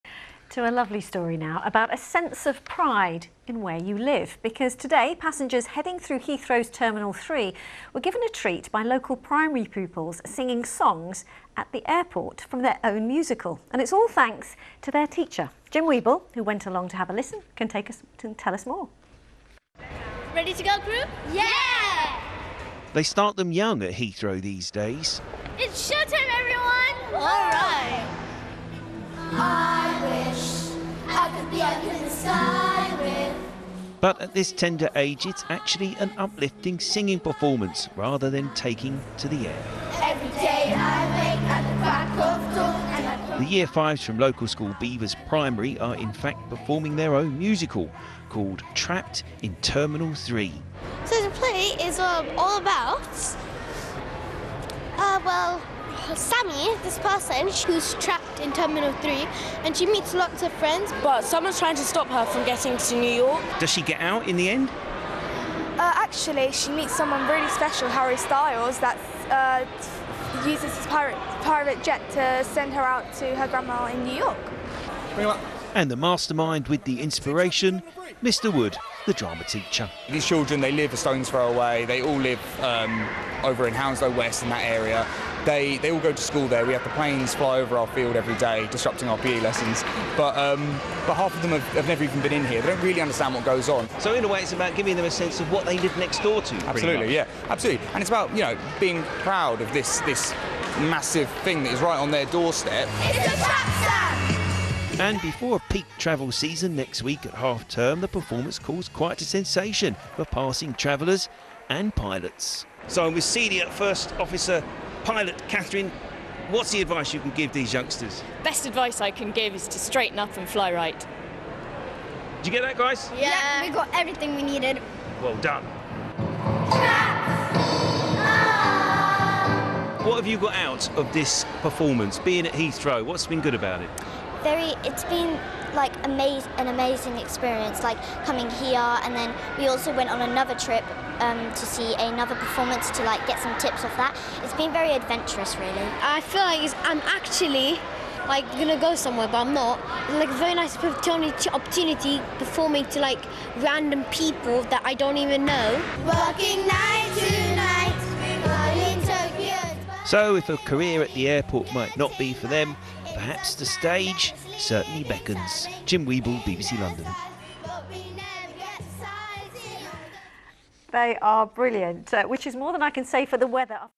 Children put on musical items they created at Heathrow (UK) 22/May/2024
There is a "People" playlist on the channel if you are interested.